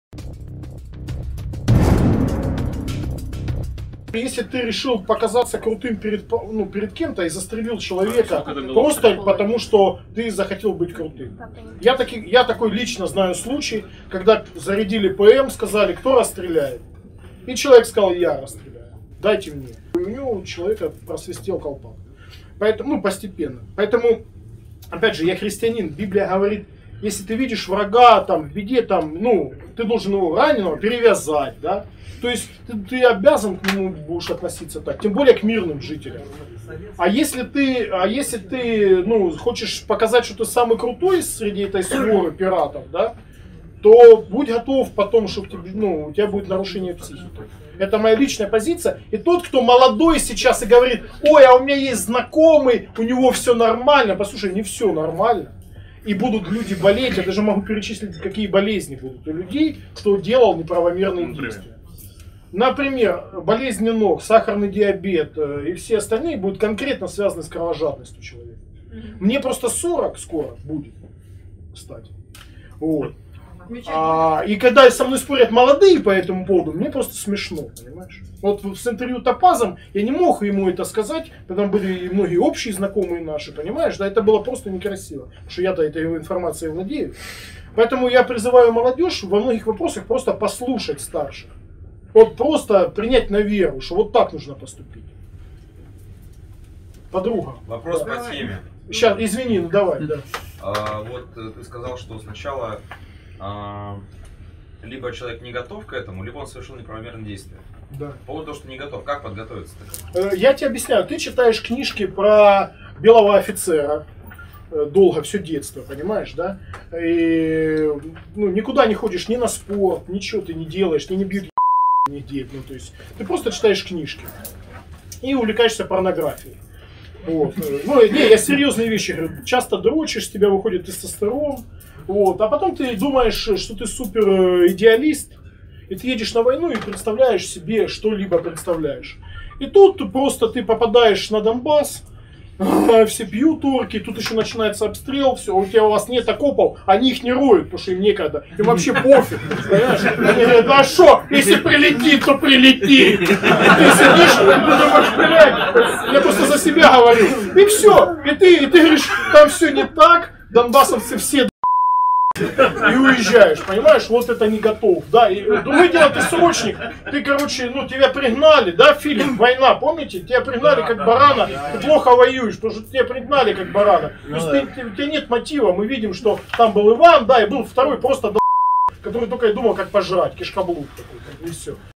«Слушать старших»: Владлен Татарский о милосердии к врагу
Владлен Татарский — писатель, блогер, ополченец — на презентации в книжной лавке «Листва»: о болезнях на войне и неготовности к войне.